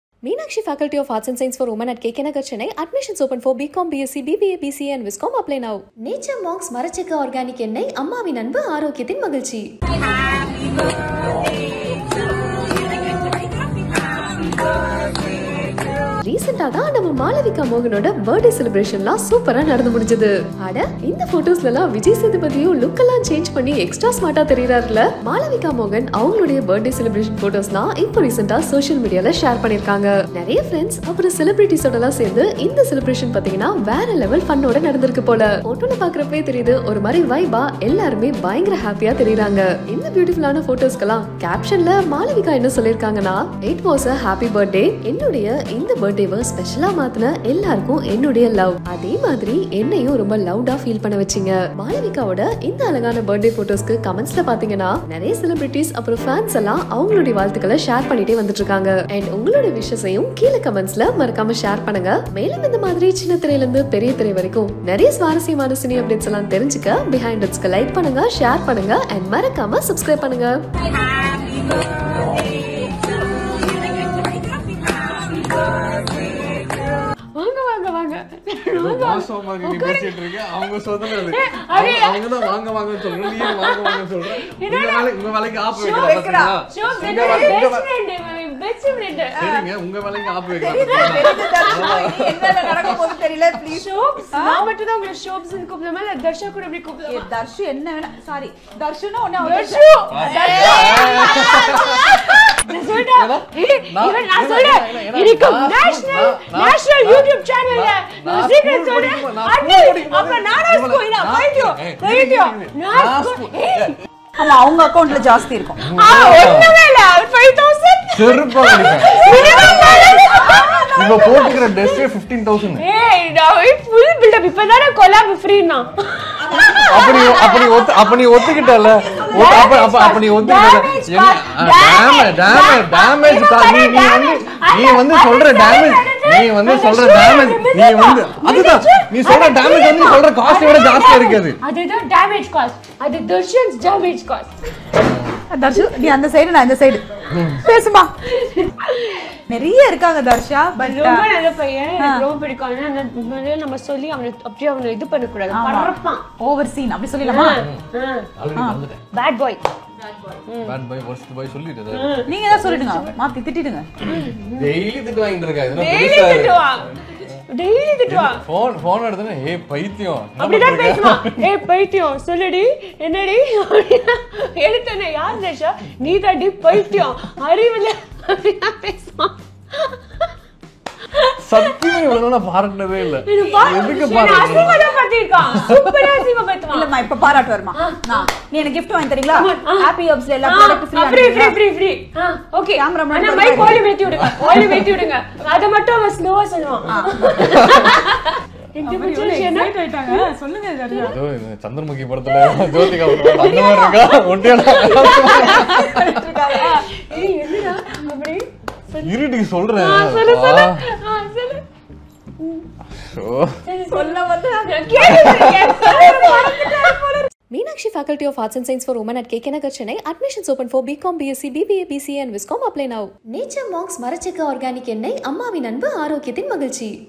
Malavika, இங்க என்ன எதிர்பாக்கலல்ல😍 Sweet Surprise கொடுத்த Vijay Sethupathi ❤️
மாளவிகா மோகன் பிறந்தநாள் விழாவில் விஜய் சேதுபதி உள்ளிட்ட பல நட்சத்திரங்கள் கலந்து கொண்டனர்.

Meenakshi college description -